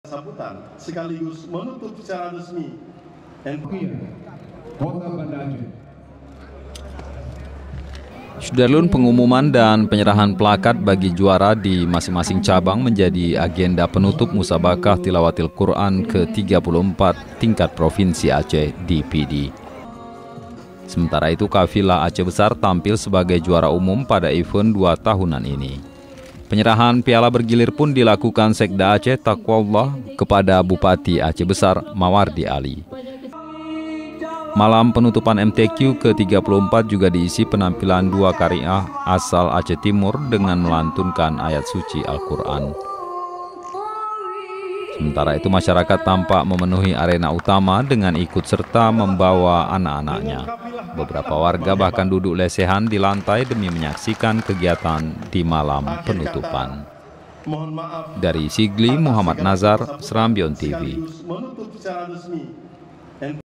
Masyarakat Padati Malam Penutupan MTQ, Aceh Besar Keluar Sebagai Juara Umuum
SERAMBITV.COM, SIGLI - Pengumuman dan penyerahan plakat bagi juara di masing-masing cabang, menjadi agenda penutup Musabaqah Tilawatil Quran (MTQ) ke-34 tingkat Provinsi Aceh di Pidie, Sabtu (28/9/2019) malam.Sementara itu, Kafilah Aceh Besar tampil sebagai juara umum pada even dua tahunan ini. Penyerahan piala bergilir pun dilakukan Sekda Aceh, Taqwallah, kepada Bupati Aceh Besar, Mawardi Ali.Malam penutupan MTQ ke-34 juga di isi penampilan dua qariah asal Aceh Timur dengan melantunkan ayat suci Alquran.Sementara itu, masyarakat tampak memenuhi arena utama dengan ikut serta membawa anak-anaknya. Beberapa warga bahkan duduk lesehan di lantai demi menyaksikan kegiatan di malam penutupan.